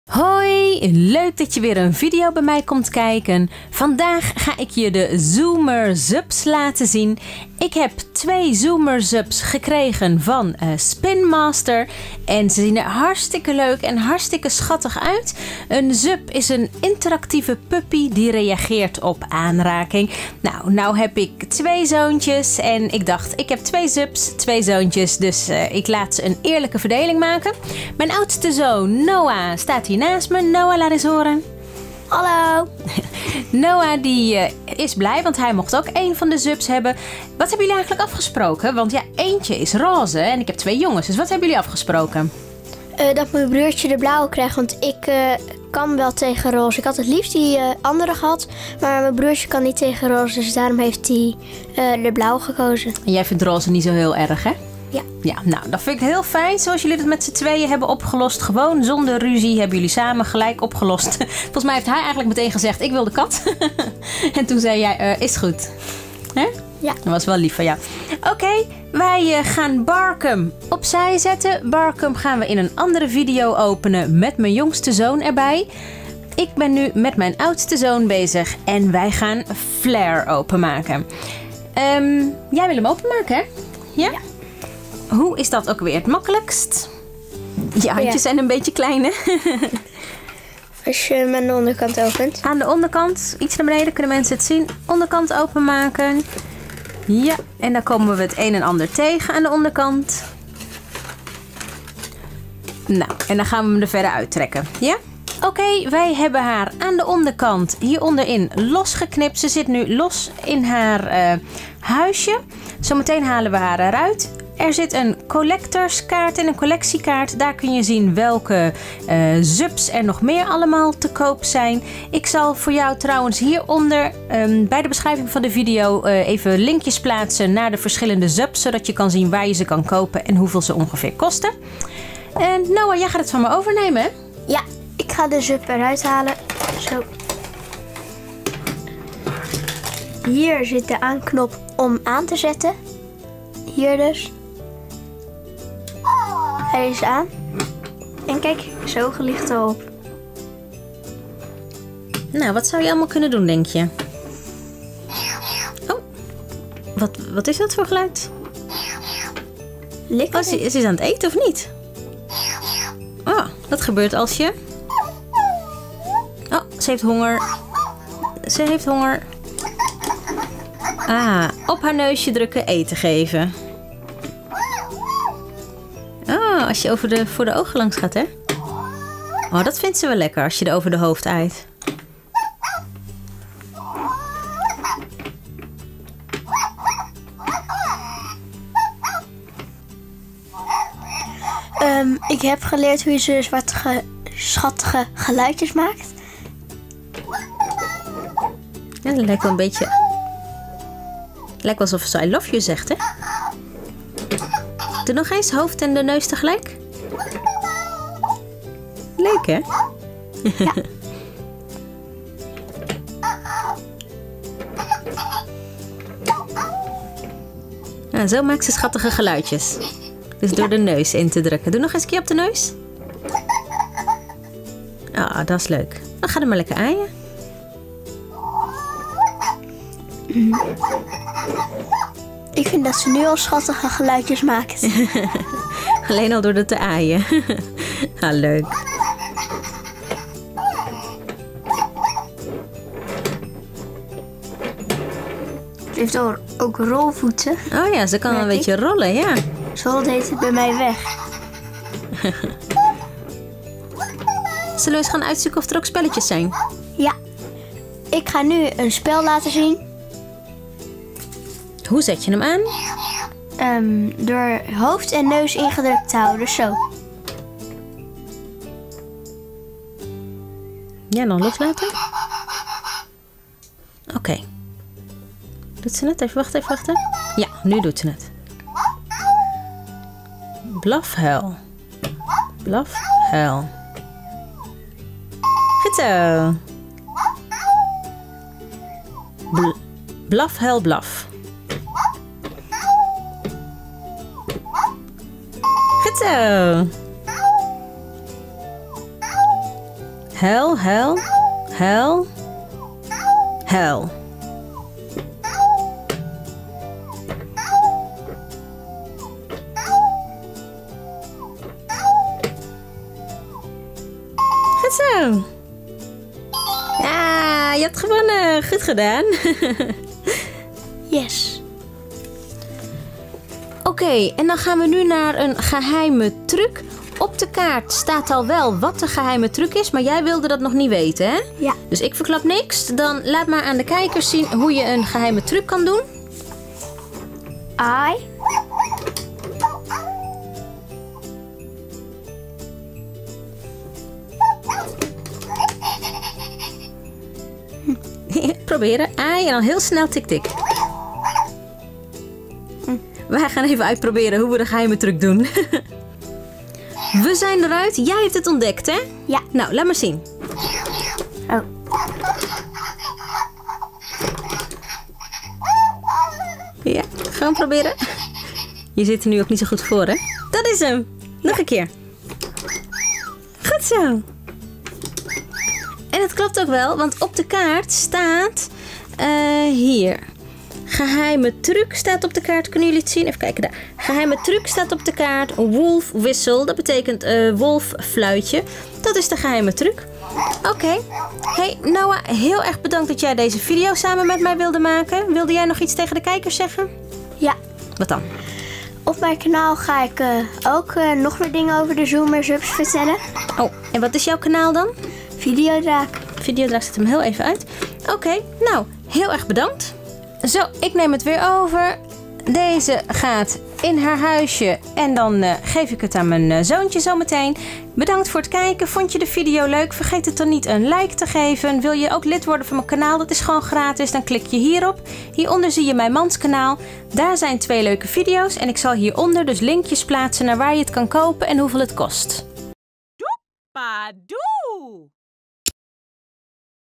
Zoomer Zupps Tiny Pups unboxing & review Nederlands - Ik heb Flare gekregen van Spin Master
Ik heb twee Zoomer Zupps gekregen van Spin Master. Flare en Barkhem. Mijn oudste zoon van 9 jaar "Video draak" helpt me bij het uitpakken van deze video.
Mijn Snapchataccount (accountnaam: JufJannieApps)
Zoomer Zupps mini pups zijn er in verschillende soorten en kleuren. Ze kunnen ook “I love you” blaffen.
Hoe doe je het geheime trucje? Mijn zoontje laat je zien hoe de secret trick werkt.
Wil je mijn nieuwste filmpjes niet missen?

Volg me op Twitter: @JufJannieApps.

Wil je meer ideeën?
Easy crafting fun and tutorials.